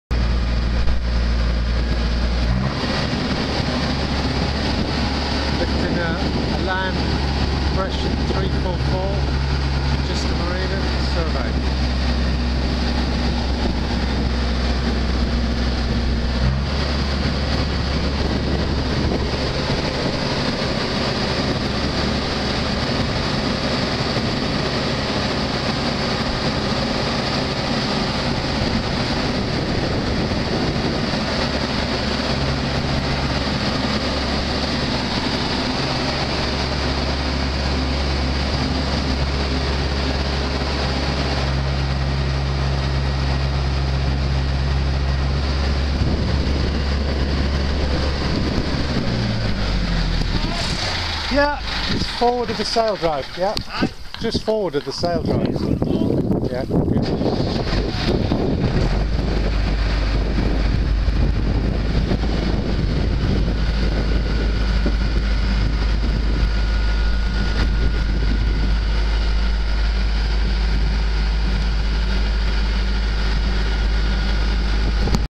Lifting an Elan 344 for survey
Survey report on an Elan 344 in Chichester Marina by RP Marine Surveys